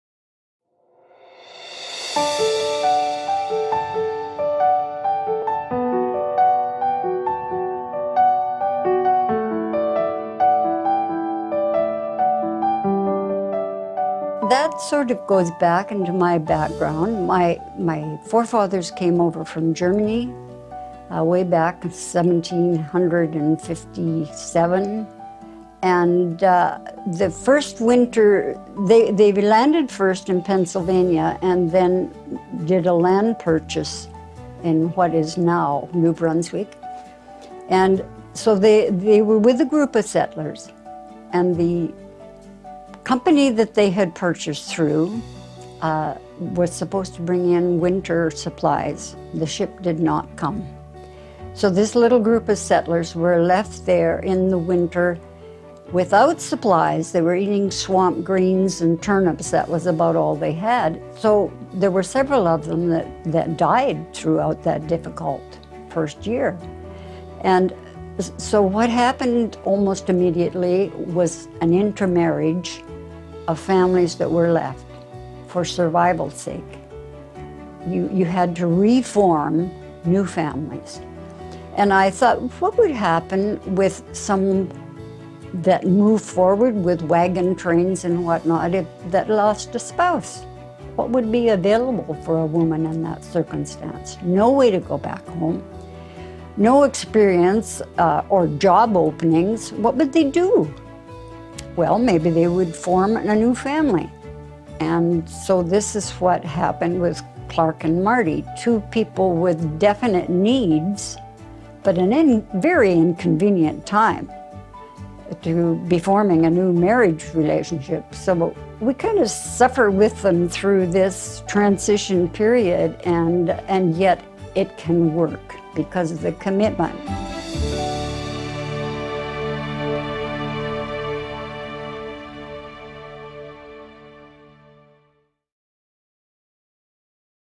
What inspired Janette Oke to write her book?
You can see this and more on any Edify Films When Calls the Heart Collectors Edition.
Just go to WhenCallsTheHeart.com to order.